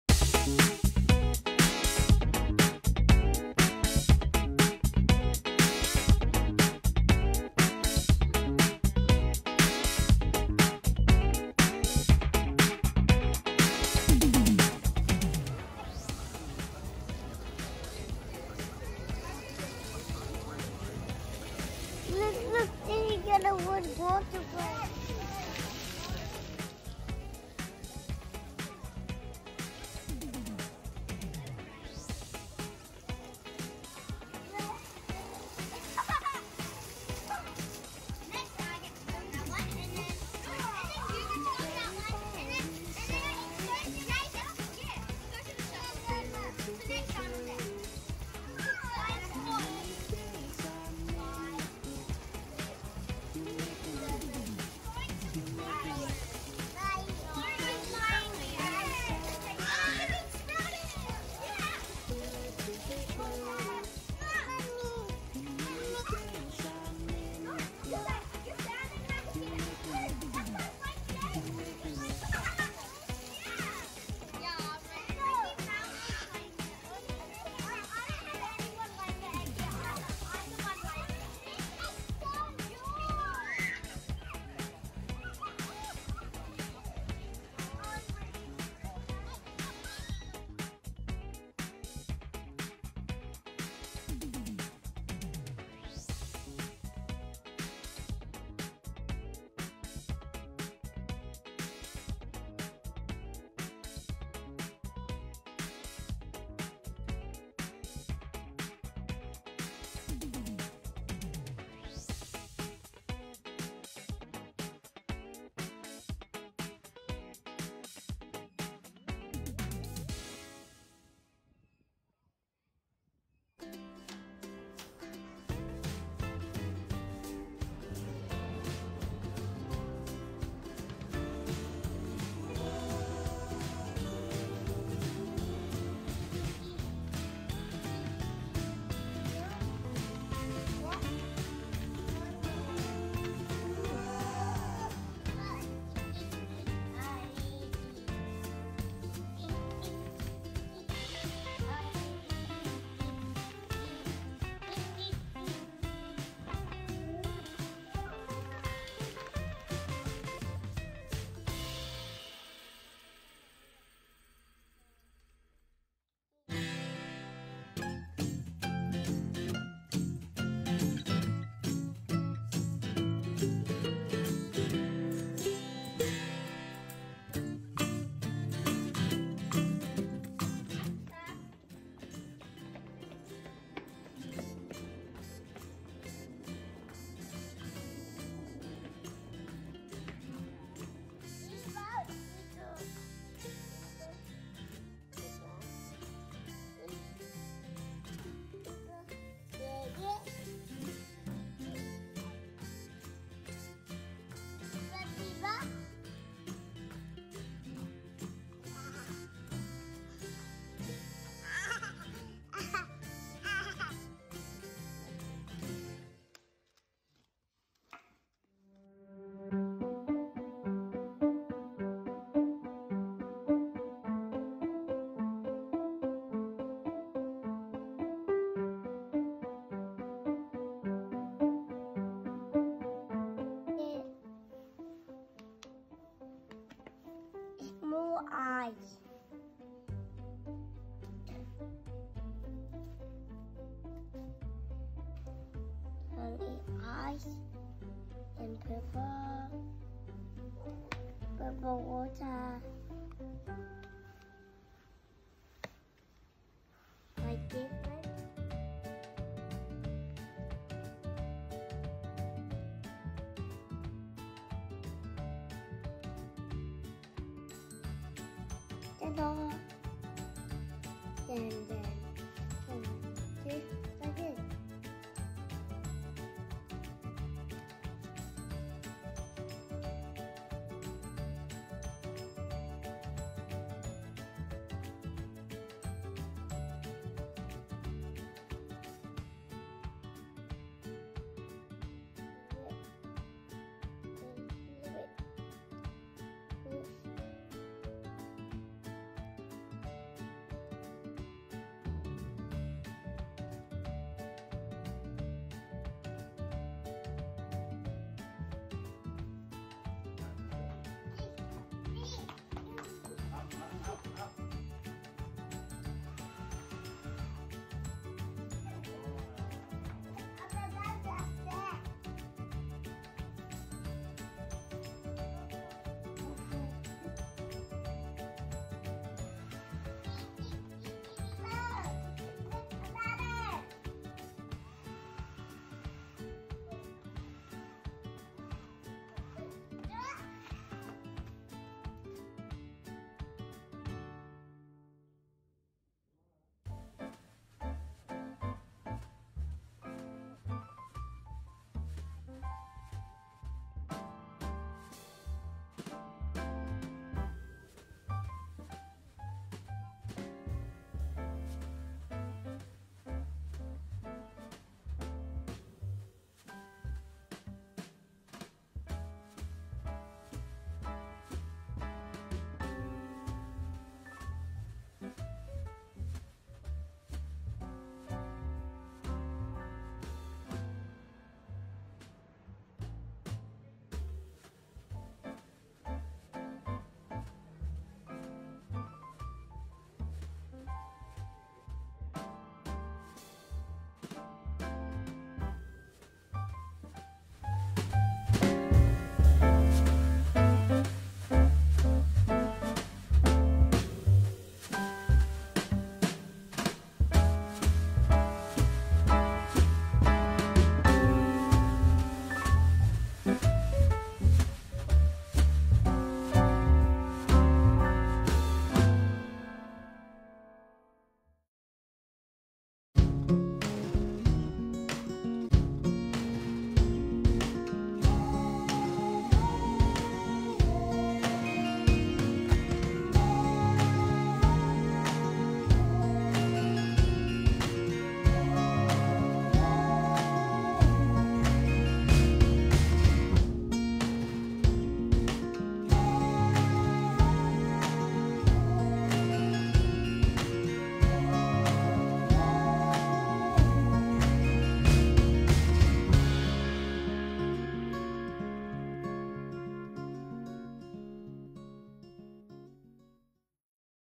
Life in Australia | Summer w my Children - water play, Ice Cream Play, Dinner at Italian Restaurant
I am a mother of two children.
My channel mainly about
1. what to do in Adelaide
2. where to go (playgrounds) in Adelaide
3. what to eat (with children or without children) in Adelaide.

This video is about how to have fun in Summer:
- Plant 4 Bowden Water Play & Playground
- Plant 4 Bowden Nutrition Republic (Vegan Cafe)
- Home playing Wooden Animal Threading Set
- Home playing Ice + Cream (Shaving Foam) Play
- Home Festive Decorations for Chinese New Year
- Home playing Bubble Blower (thousand bubbles per min)
- Family Dinner at Piccoli Piatti Italian Restaurant
- Adelaide Aquatic Centre

—- Social Media —-